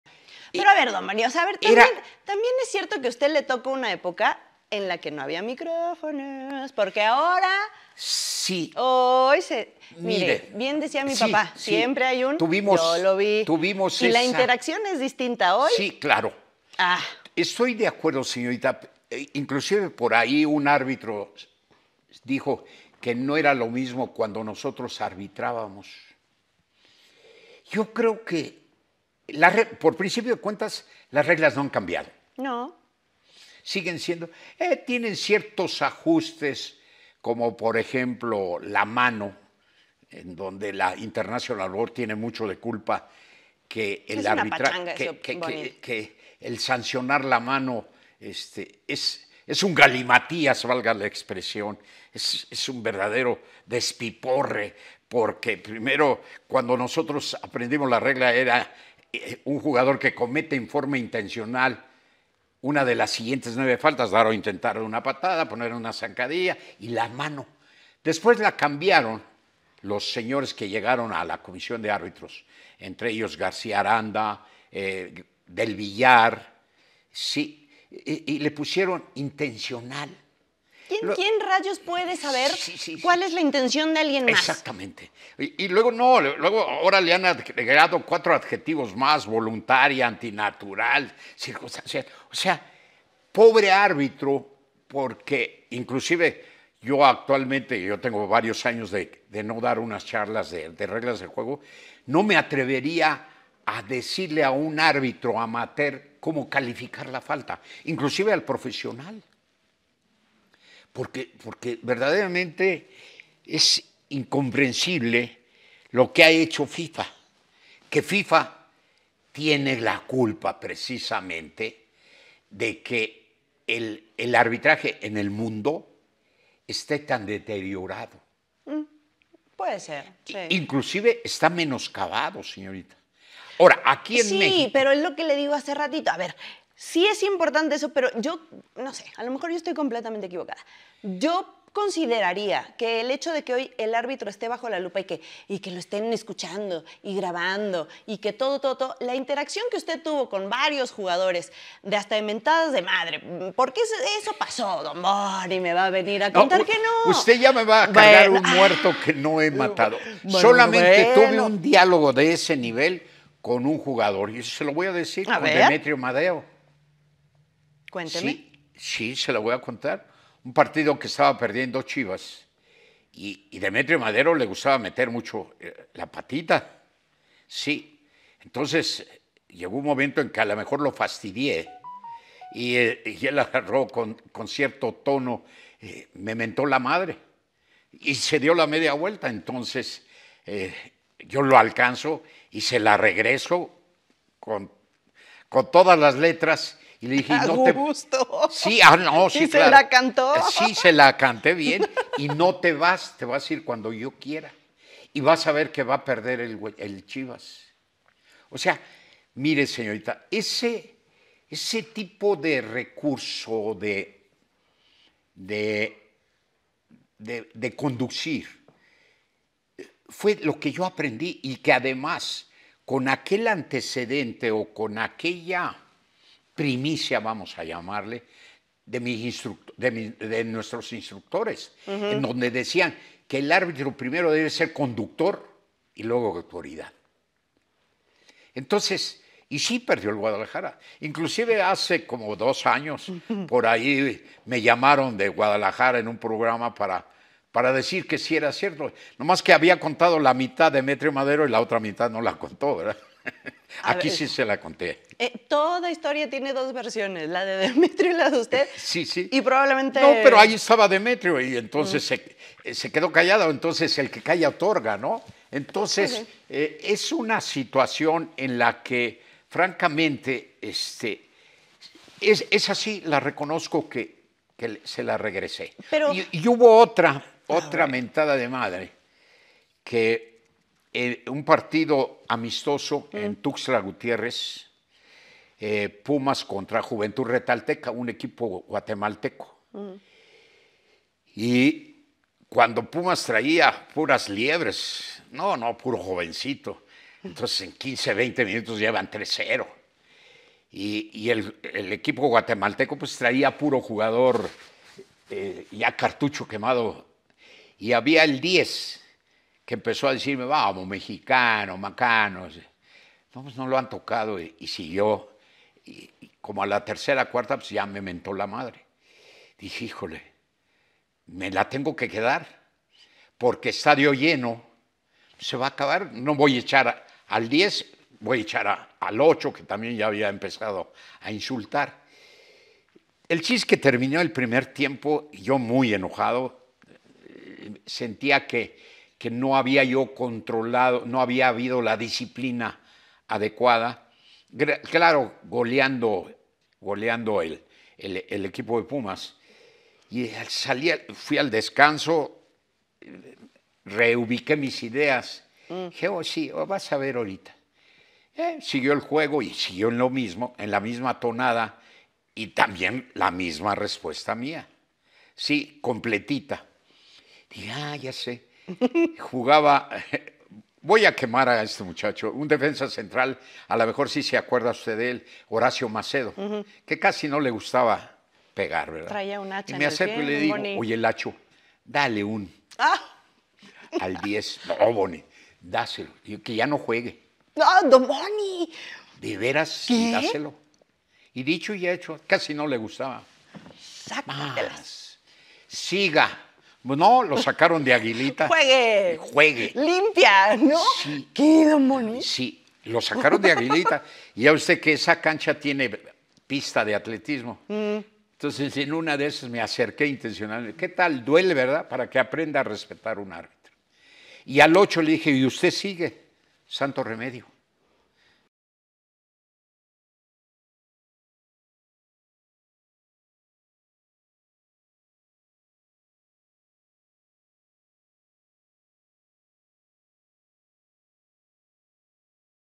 Bonifacio Núñez explotó contra la FIFA | El Marcador
BonifacioNúñez, confesó lo difícil y a la vez satisfactoria que fue la época sin micrófonos ni VAR en el fútbol profesional.